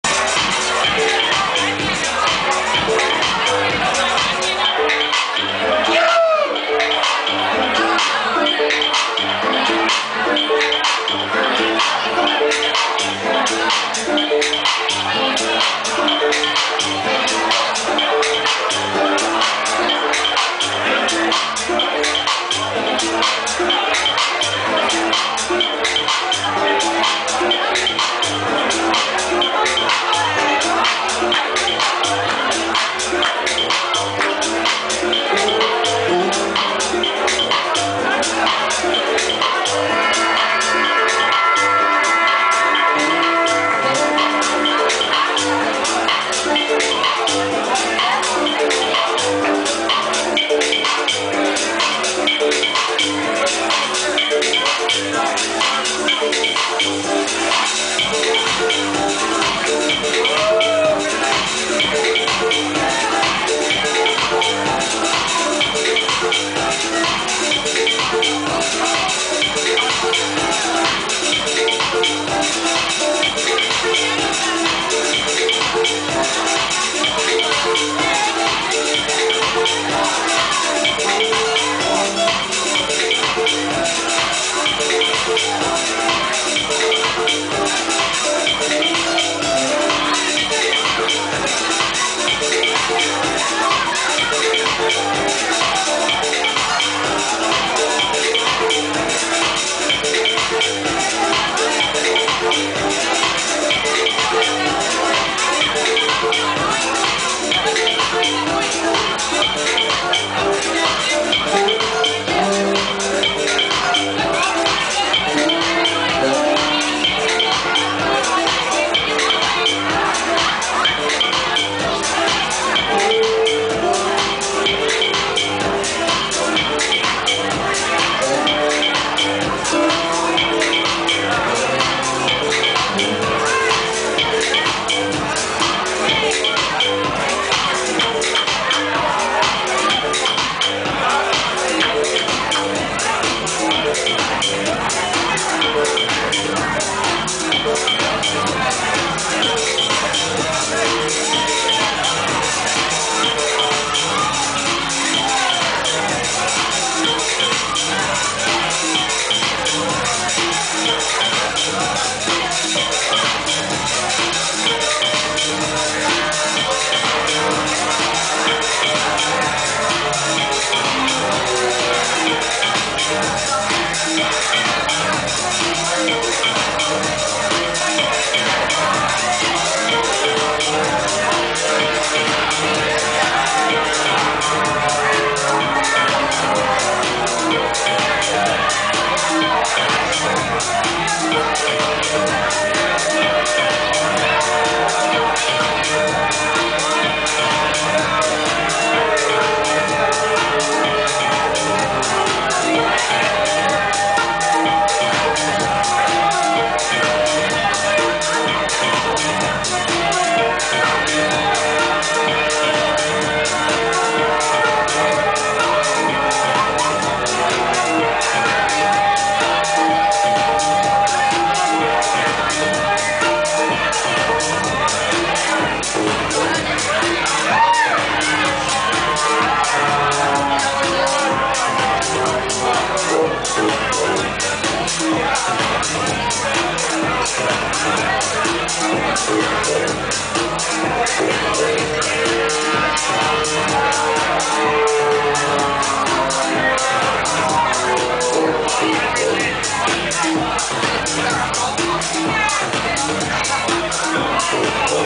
Peter Kruder @ Halo 3.24.09
Ever wanted to stand next to Peter Kruger as he spins?  Well, about 100 of us got that oportunity as he came through on his way to Winter Music Conference.  Here is a four minutes next to the decks as he spins at Halo in Atlanta.